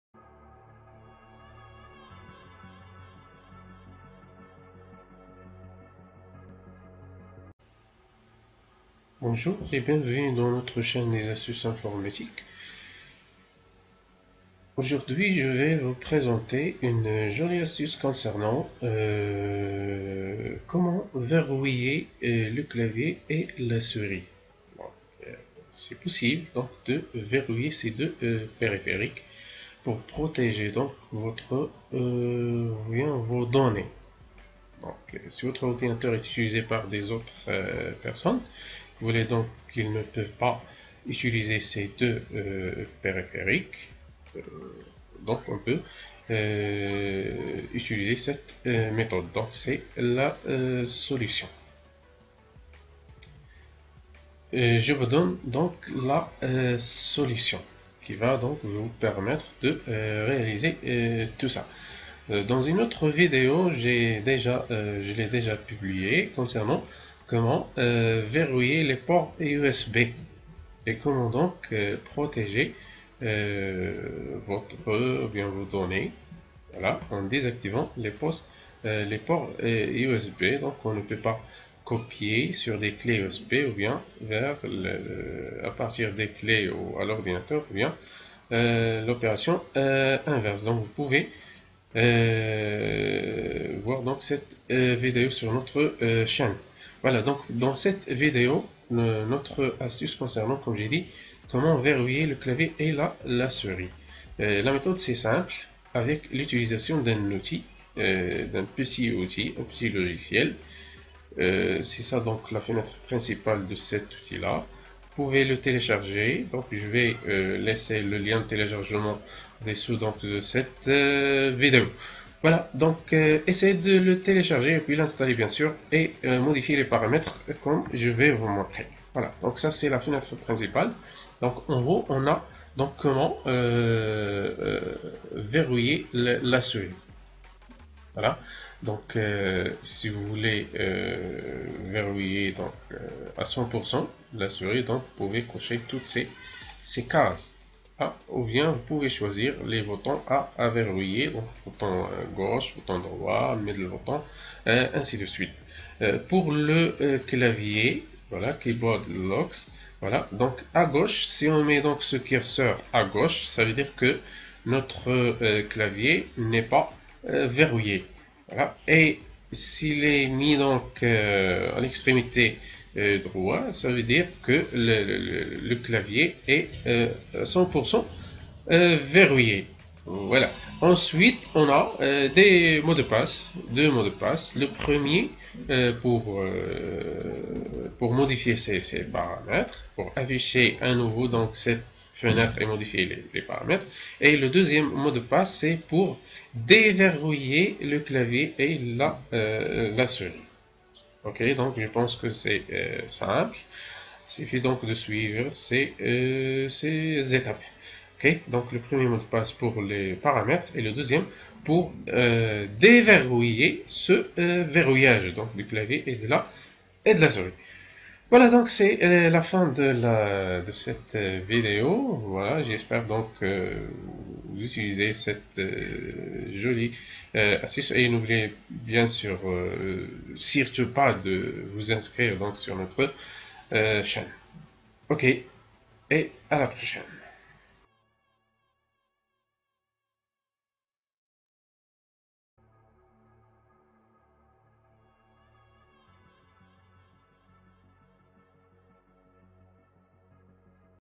Astuces PC: Comment verrouiller le clavier et la souris de votre PC quand vous n'êtes pas là
Lien du téléchargement goo.gl/0Q1Lwt